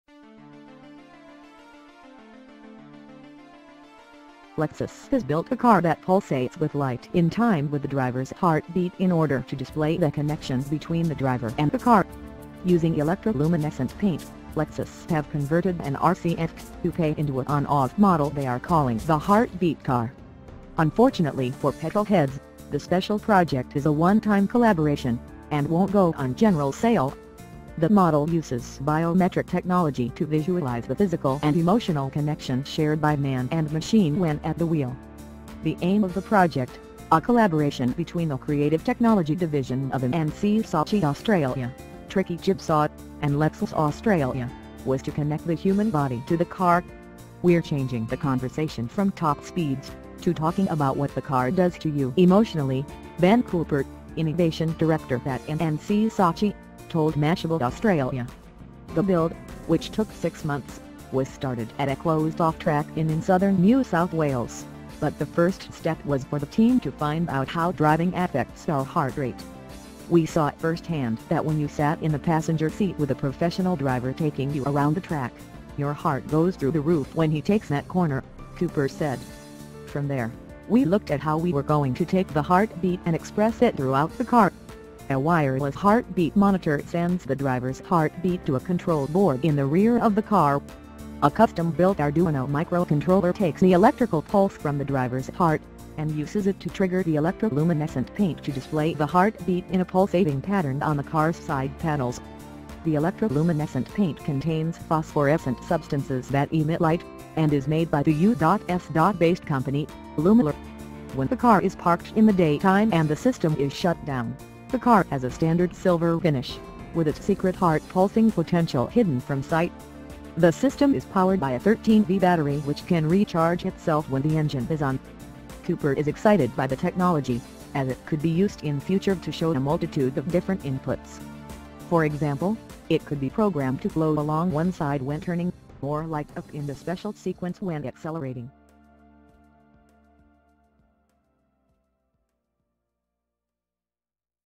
Lexus builds a car with a human HEARTBEAT
Lexus has built a car that pulsates with light in time with the driver's heartbeat in order to display the connection between the driver and the car.
Using electro-luminescent paint, Lexus have converted an RC F coupe into a on-off model they are calling the 'heartbeat car'.
Unfortunately for petrol heads, the special project is a one-time collaboration, and won't go on general sale.
The model uses biometric technology to visualise the physical and emotional connection shared by man and machine when at the wheel.
The aim of the project, a collaboration between the creative technology division of M&C Saatchi Australia, Tricky Jigsaw, and Lexus Australia, was to connect the human body to the car.
'We're changing the conversation from top speeds, to talking about what the car does to you emotionally,' Ben Cooper, innovation director at M&C Saatchi, told Mashable Australia.
The build, which took six months, was started at a closed-off track in in southern New South Wales, but the first step was for the team to find out how driving affects our heart rate.
'We saw firsthand that when you sat in the passenger seat with a professional driver taking you around the track, your heart goes through the roof when he takes that corner,' Cooper said.
'From there, we looked at how we were going to take the heartbeat and express it throughout the car.'
A wireless heartbeat monitor sends the driver's heartbeat to a control board in the rear of the car.
A custom-built Arduino micro-controller takes the electrical pulse from the driver's heart, and uses it to trigger the electro-luminescent paint to display the heartbeat in a pulsating pattern on the car's side panels.
The electro-luminescent paint contains phosphorescent substances that emit light, and is made by the U.S.-based company, Lumilor.
When the car is parked in the day time and the system is shut down, the car has a standard silver finish, with its secret heart pulsing potential hidden from sight.
The system is powered by a 13V battery which can recharge itself when the engine is on.
Cooper is excited by the technology, as it could be used in future to show a multitude of different inputs.
For example, it could be programmed to glow along one side when turning, or light up in a special sequence when accelerating.